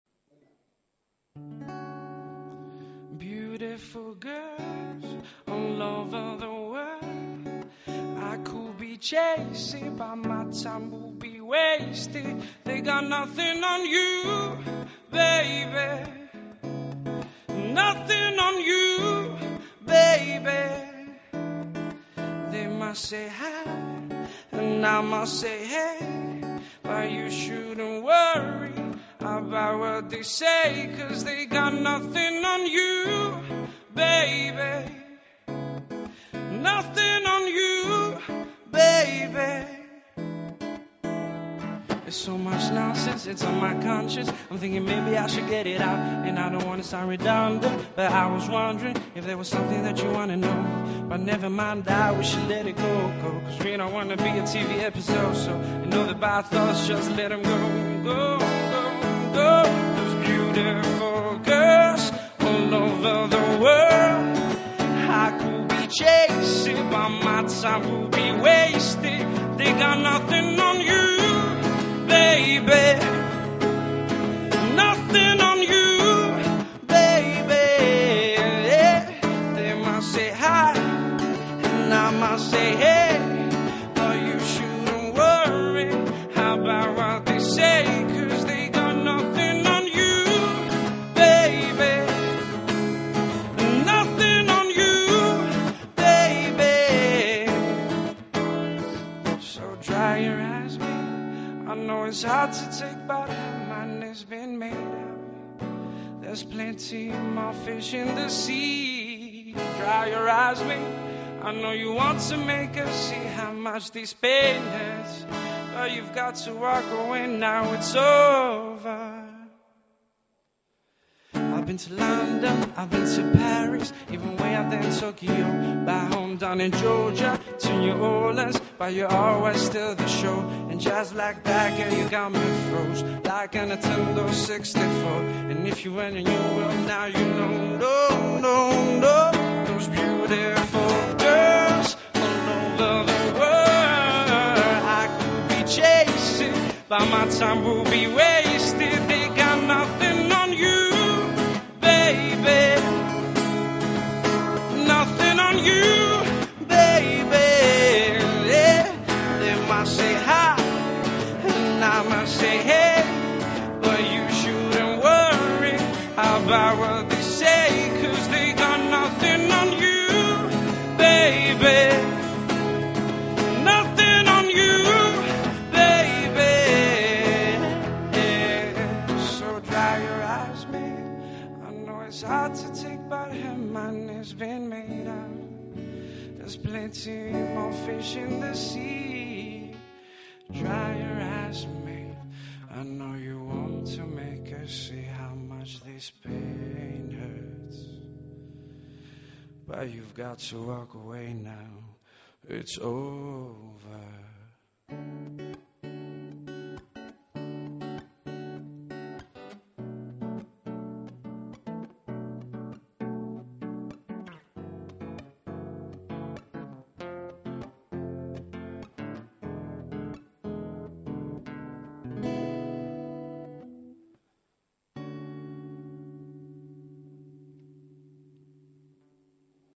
Σταύρος Μιχαλακάκος at Mad Day Live (part 1)
Εβδομάδα Ελληνικής Μουσικής με Σταύρο Μιχαλακάκο at Mad Day Live, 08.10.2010.